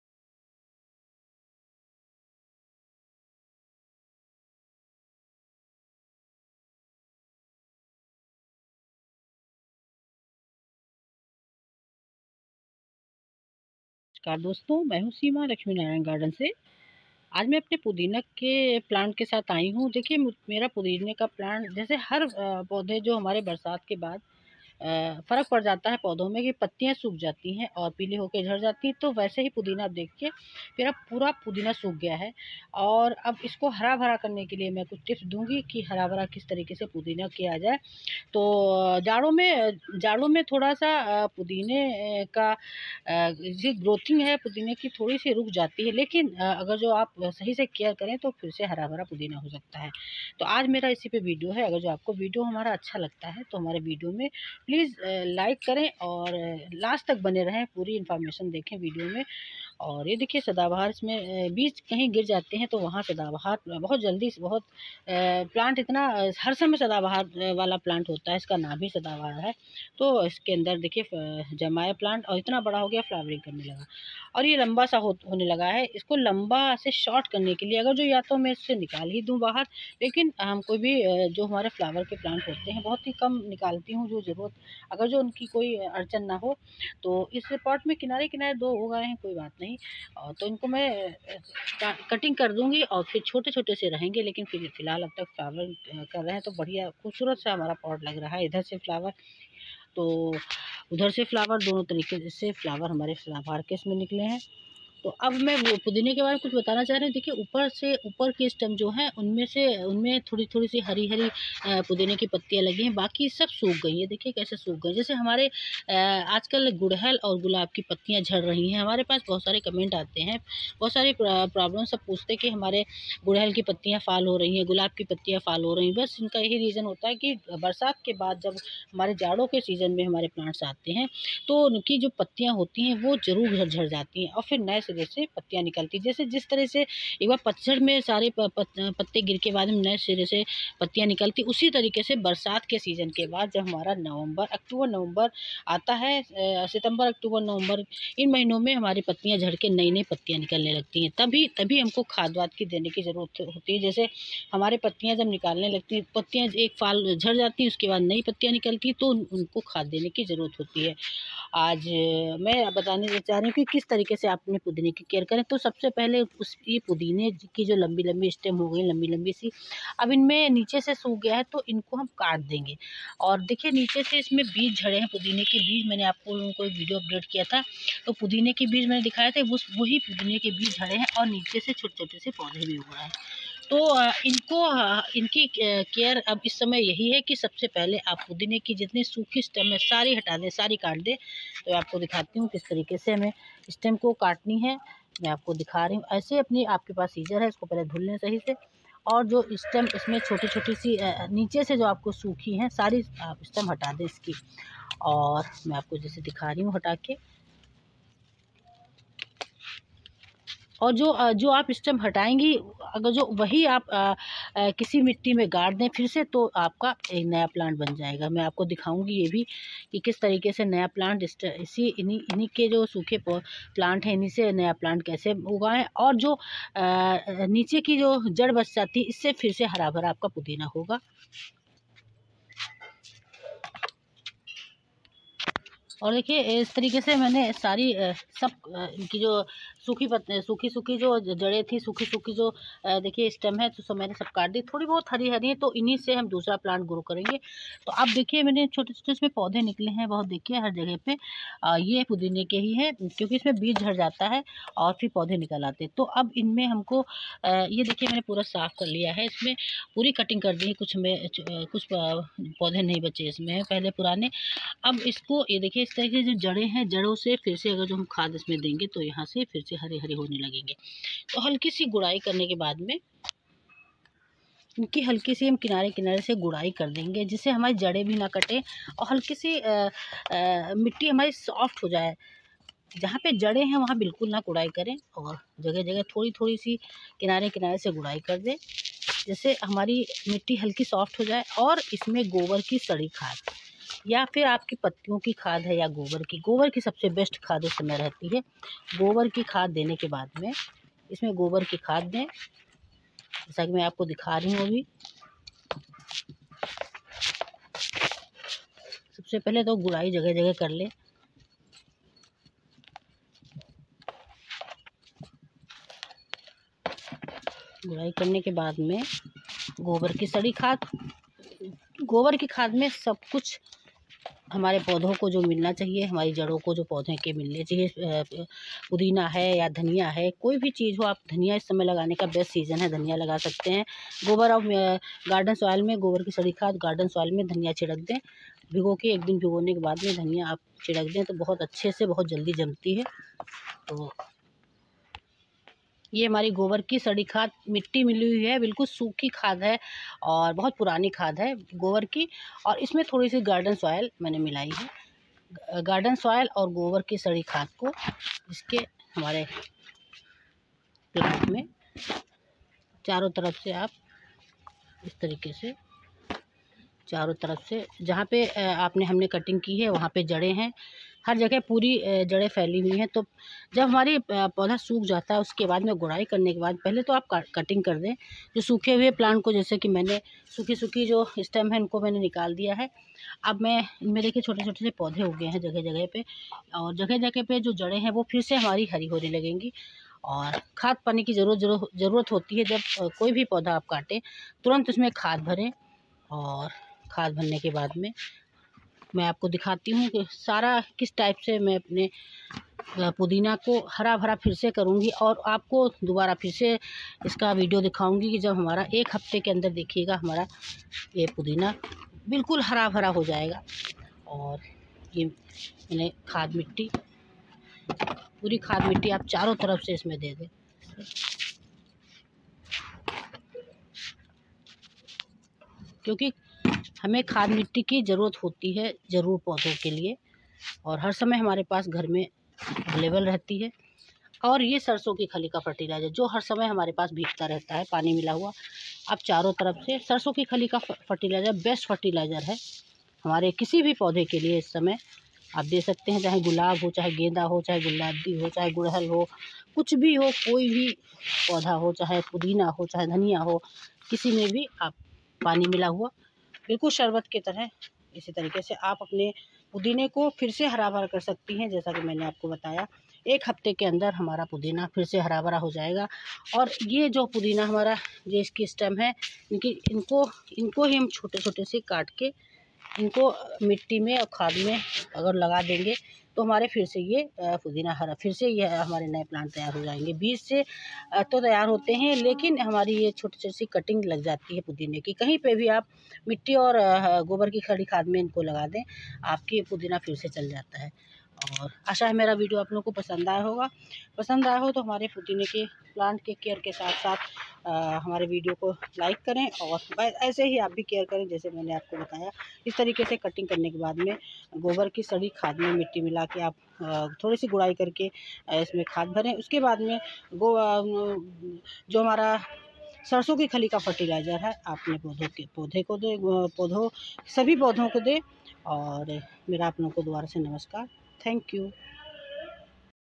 सूखे हुए पुदीने को फिर से हरा-भरा करने के लिए कुछ नए टिप्स देखें हमारे नए वीडियो में✌✌😇😇
बरसात के सीजन के बाद पुदीने के प्लांट को  फिर से कैसे हरा भरा करें देखें ,कुछ टिप्स कुछ टिप्स जो मैंने अपने इस नए वीडियो में दिखाएं बताएं हैं पूरा वीडियो देख कर अपने पुदीने को फिर से हरा-भरा करें।